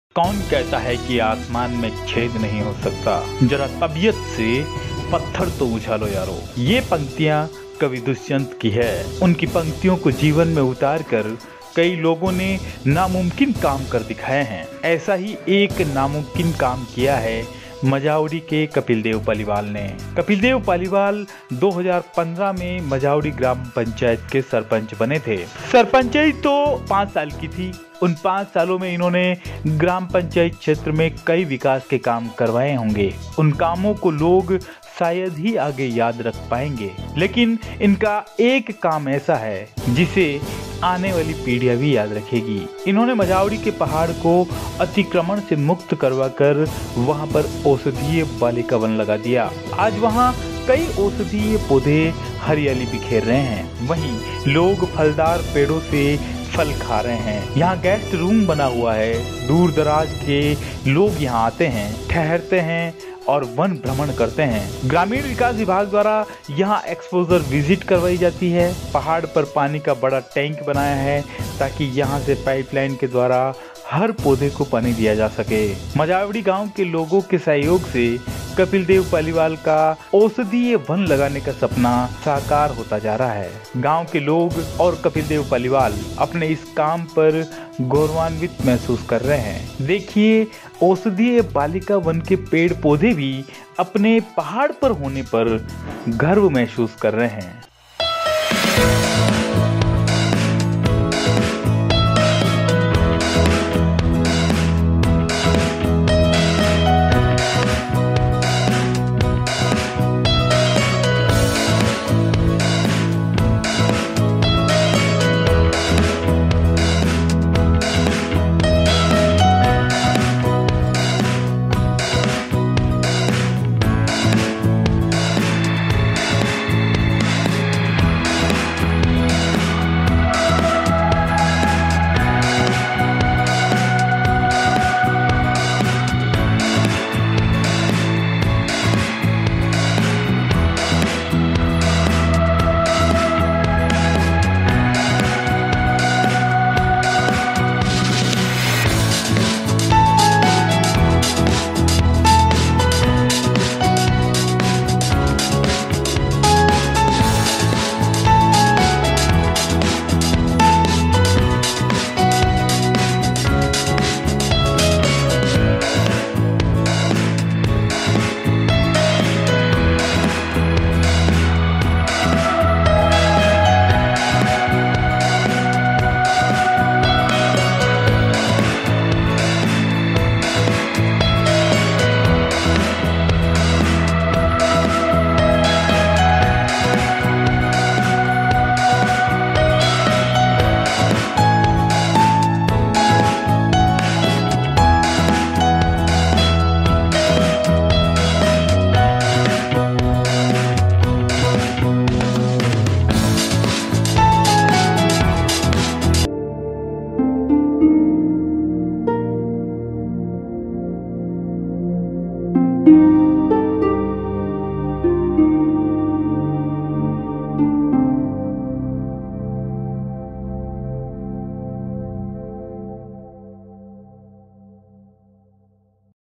ऐसा हो गया है औषधीय बालिका वन ! Majawari I Gogunda I Udaipur I
Welcome In Daily Rajasthan.
कौन कहता है कि आसमान में छेद नहीं हो सकता।
जरा तबियत से पत्थर तो उछालो यारों।
ये पंक्तियां कवि दुष्यंत की है। उनकी पंक्तियों को जीवन में उतार कर कई लोगों ने नामुमकिन काम कर दिखाए है। ऐसा ही एक नामुमकिन काम किया है मजावड़ी के कपिलदेव पालीवाल ने। कपिलदेव पालीवाल 2015 में मजावड़ी ग्राम पंचायत के सरपंच बने थे। सरपंचाई तो 5 साल की थी। उन 5 सालों में इन्होंने ग्राम पंचायत क्षेत्र में कई विकास के काम करवाए। उन कामों को लोग शायद ही याद रख पायेंगे लेकिन इनके एक काम को आने वाली पीढ़ियां भी याद रखेगी। इन्होंने मजावड़ी के पहाड़ को अतिक्रमण से मुक्त करवाकर वहां औषधीय बालिका वन लगा दिया। आज वहां कई औषधीय पौधे हरियाली बिखेर रहे है वहीं लोग फलदार पेड़ों से फल खा रहे है। यहां गेस्ट रूम बना हुआ है, दूरदराज से लोग यहां आते है, ठहरते है और वन भ्रमण करते है। ग्रामीण विकास विभाग द्वारा यहां एक्सपोजर विजिट करवाई जाती है। पहाड़ पर पानी का बड़ा टैंक बनाया है ताकि यहां से पाइपलाइन के द्वारा हर पौधे को पानी दिया जा सके। मजावड़ी गांव के लोगों के सहयोग से कपिलदेव पालीवाल का औषधीय वन लगाने का सपना साकार होता जा रहा है। गांव के लोग और कपिलदेव पालीवाल अपने इस काम पर गौरवान्वित महसूस कर रहे है। देखिए औषधीय बालिका वन के पेड़-पौधे भी अपने पहाड़ पर होने पर गर्व महसूस कर रहे है।
One such impossible thing has been done by Kapildev Paliwal of Majavadi. Kapildev Paliwal became the Sarpanch of Mazawadi Gram Panchayat in 2015. The sarpanchai was 5 years old. In those 5 years, he got many development works done in the Gram Panchayat area. People will hardly be able to remember those works, but the coming generations will also remember one of their works. He got the mountain of Mazawadi free from encroachment and planted a balika van there. Today many medicinal plants are spreading greenery there, while people are eating fruits from fruit trees. There is a guest room here, people from far away come here, stay and do forest tours. Exposure visits are conducted here by the Rural Development Department. A big water tank has been built on the mountain so that water can be given to every plant through pipeline from here. Kapildev Paliwal's dream of planting a medicinal forest is becoming a reality with the help of the people of Majavadi village. The people of the village and Kapildev Paliwal are feeling proud on this work. See, the trees and plants of the balika van are also feeling proud to be on their mountain.
"आमजन द्वारा, आमजन के लिए"